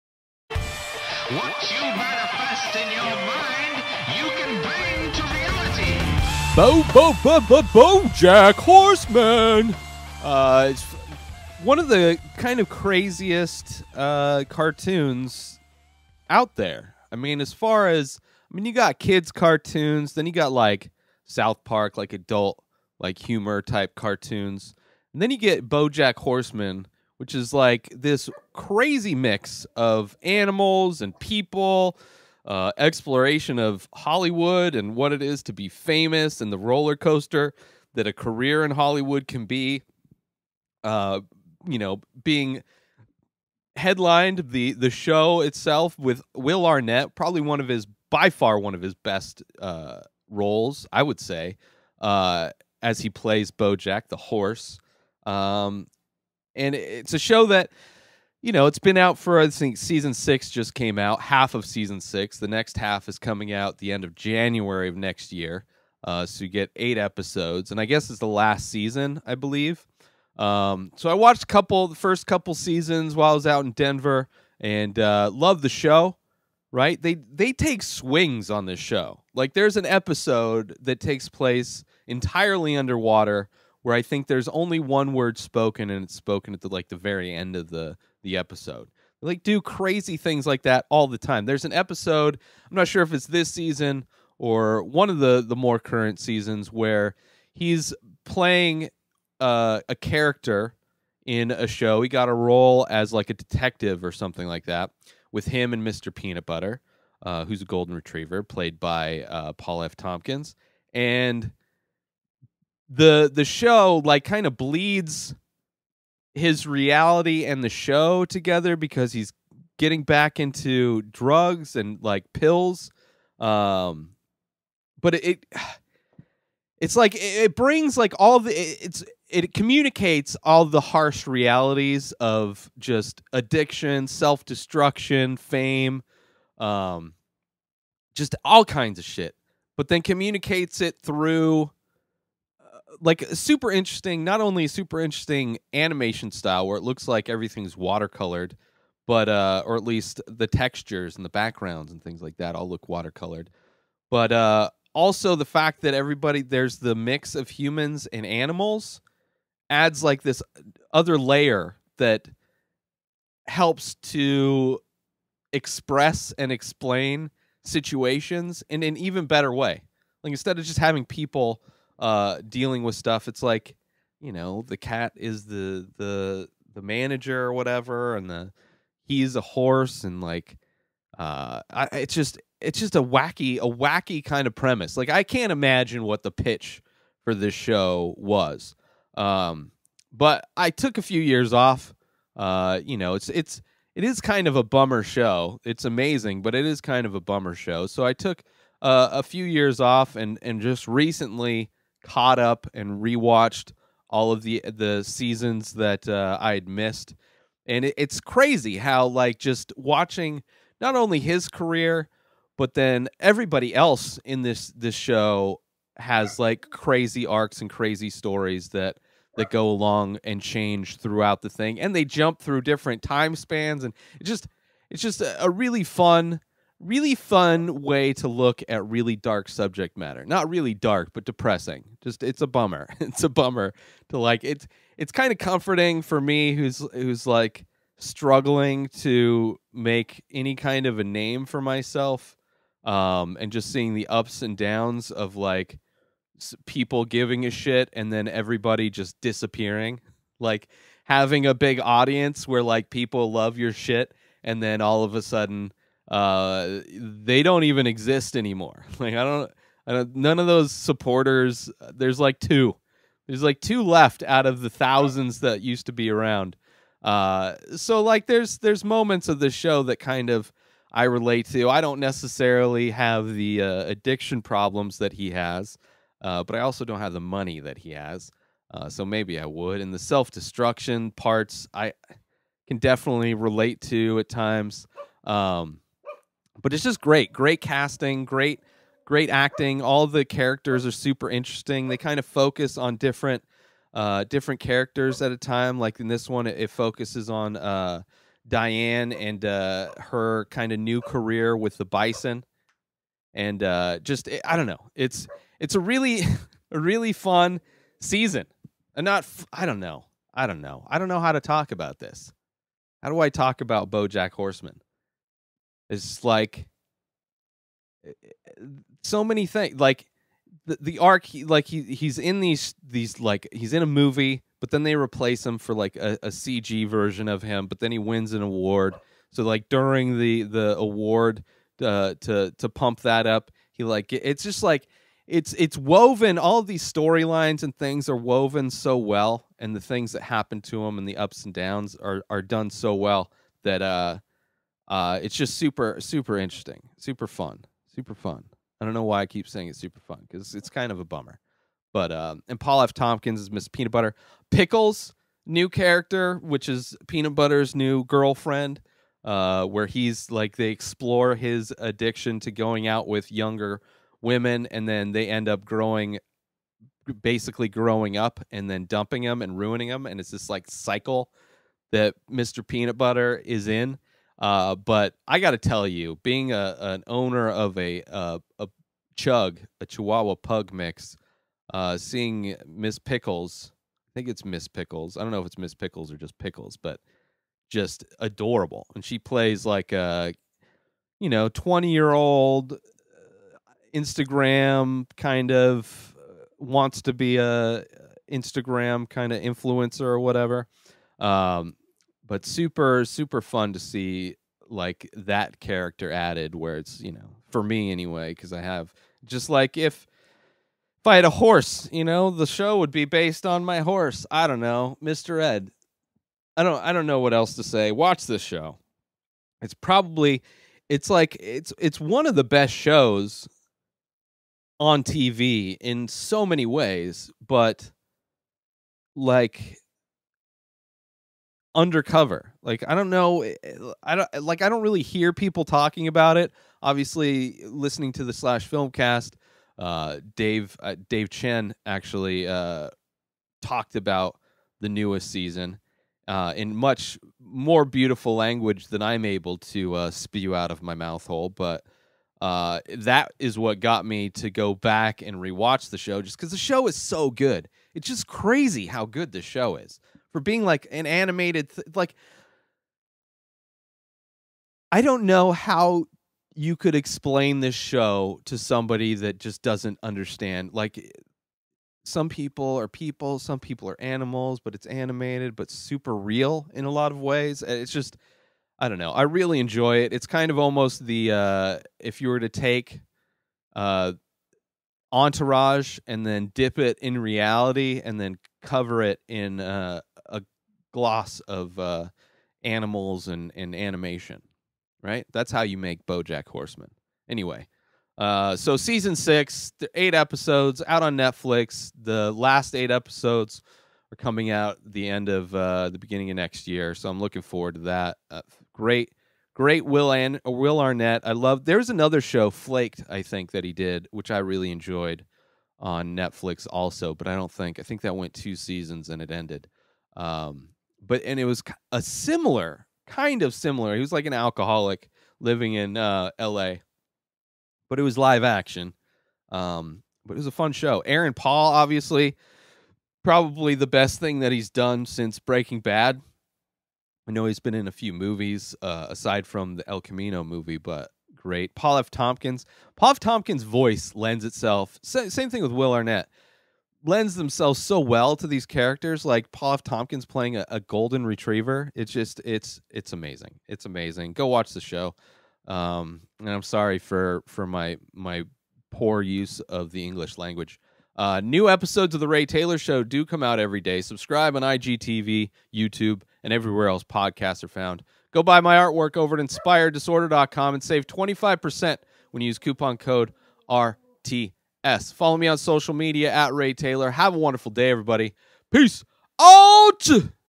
BoJack Horseman || Ray Taylor Show
Show topic: BoJack Horseman was the star of the hit television show "Horsin' Around" in the '80s and '90s, now he's washed up, living in Hollywood, complaining about everything, and wearing colorful sweaters.

Creator: Raphael Bob-Waksberg

Twitter.com/RayTaylor
Instagram.com/RayTaylorShow